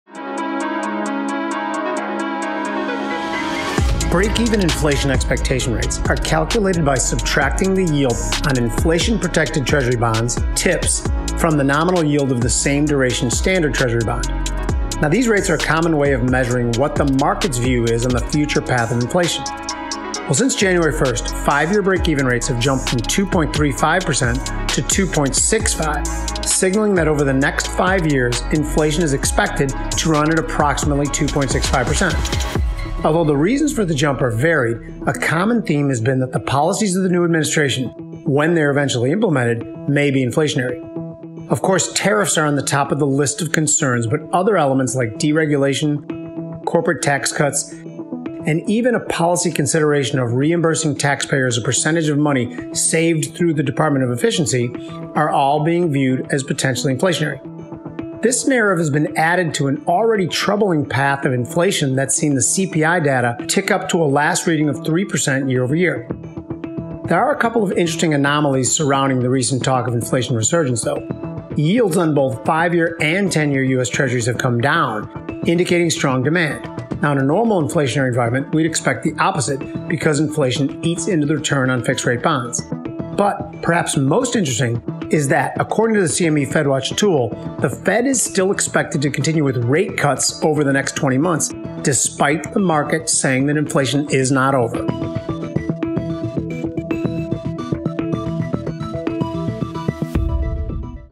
Are Breakeven Rates Signaling Higher Inflation? | Presented by CME Group
The breakeven inflation rate, a key indicator of inflation expectations, is on the rise. What factors are driving this trend, and what message is it sending to bond investors?  Presented by @cmegroup